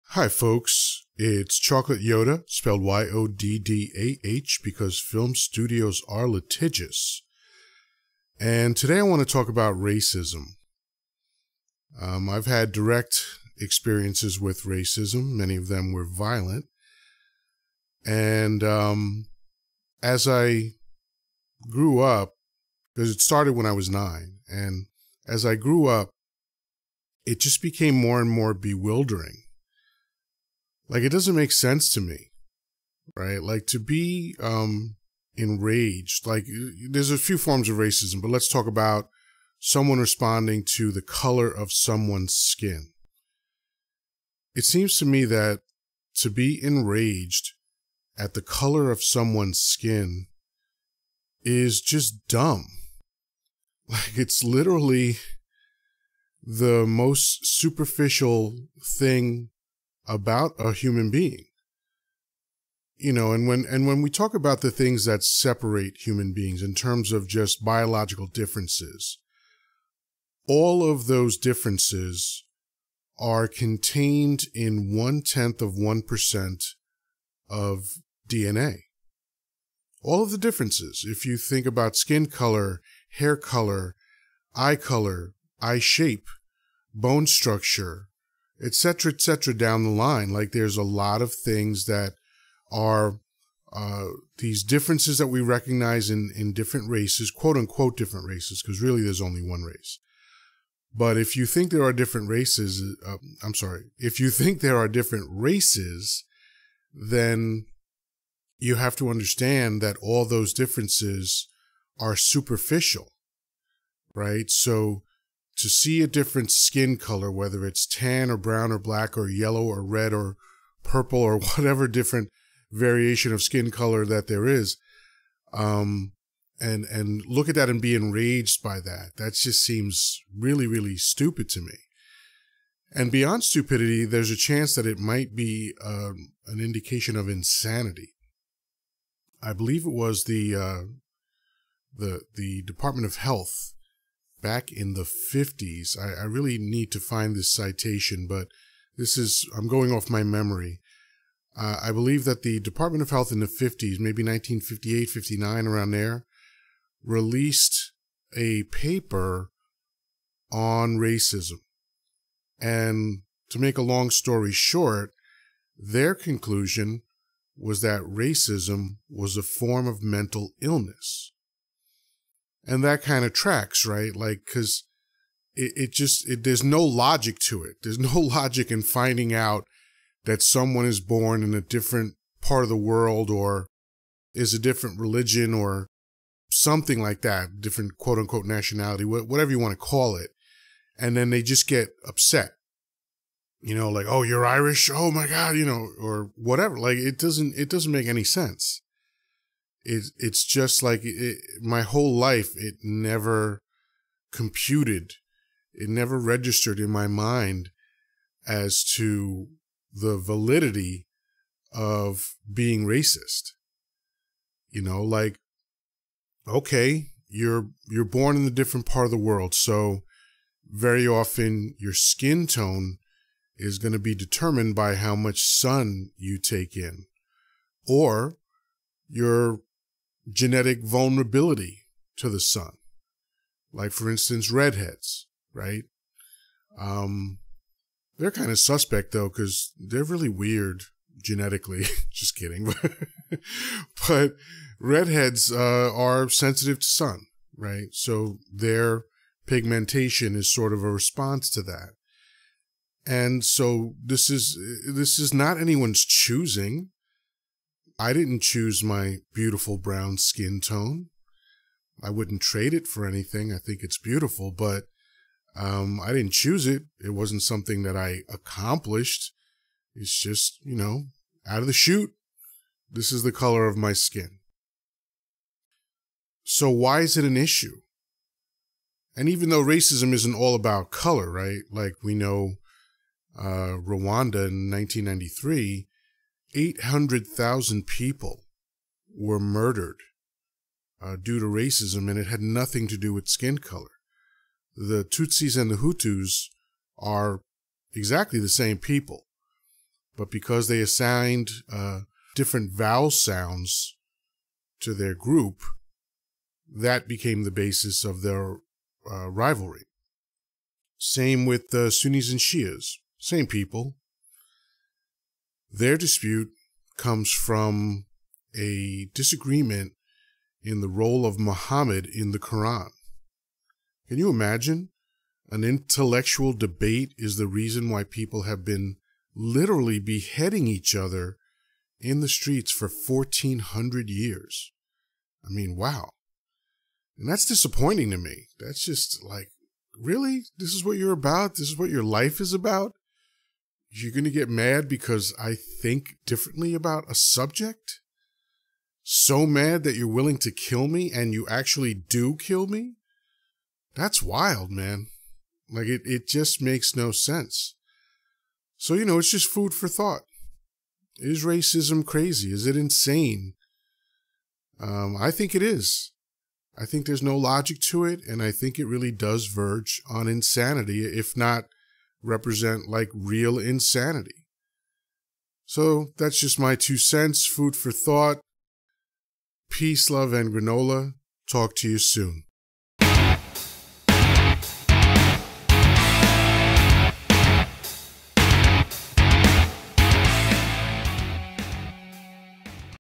Chocolate Yoddah - Is Racism Insane?
Chocolate Yoddah shares some thoughts about racism.